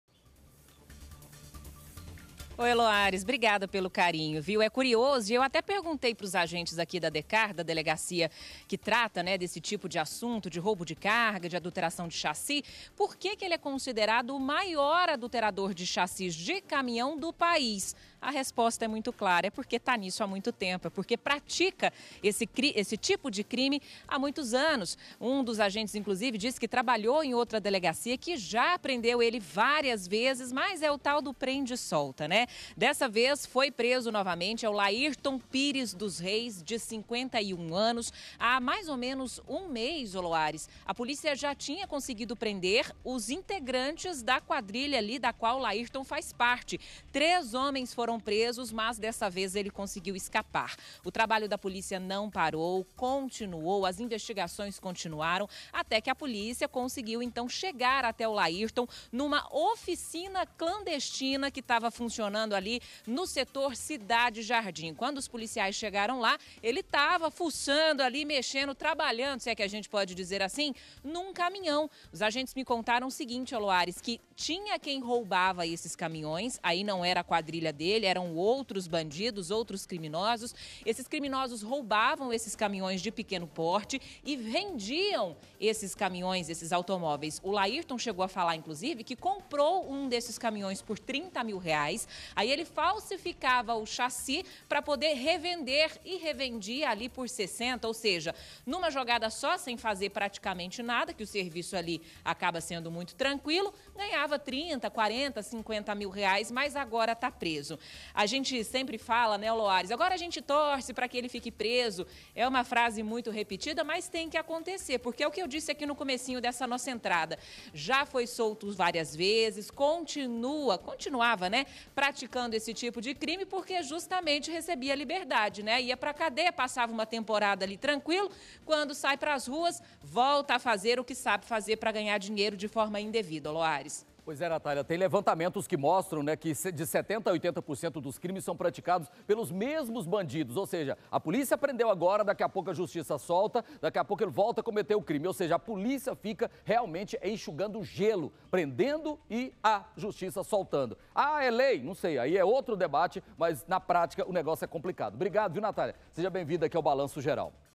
OFICINA CLANDESTINA: PRESO MAIOR ADULTERADOR DE CHASSIS DO BRASIL
MAIOR ADULTERADOR DE CHASSIS DO PAÍS É PRESO EM GOIÂNIA.